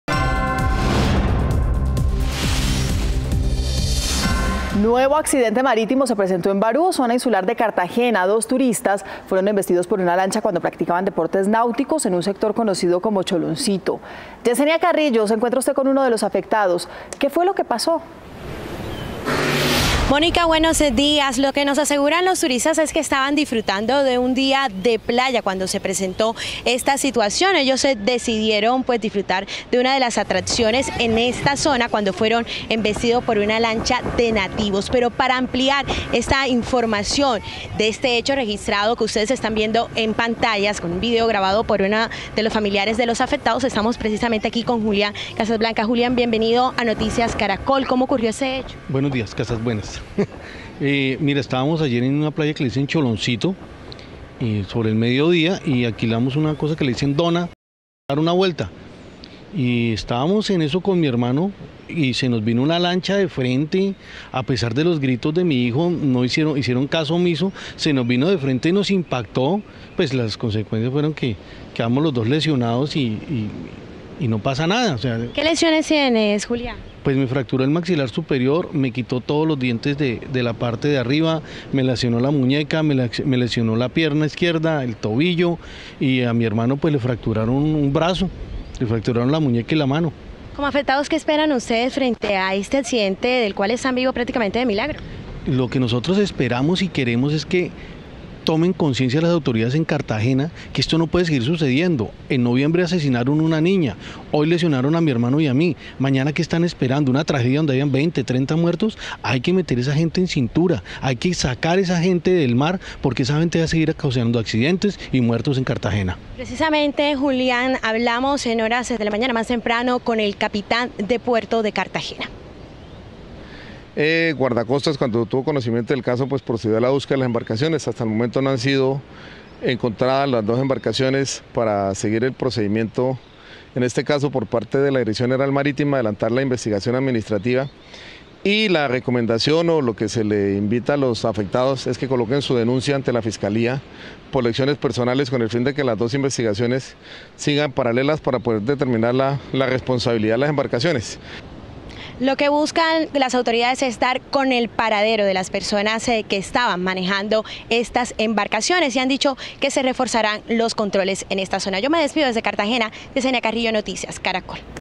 Impactante momento en que lancha atropella a dos turistas en Barú
Iban montados en un bote cuando fueron embestidos. Sufrieron varias fracturas y uno, incluso, perdió parte de los dientes. Todo quedó grabado en video.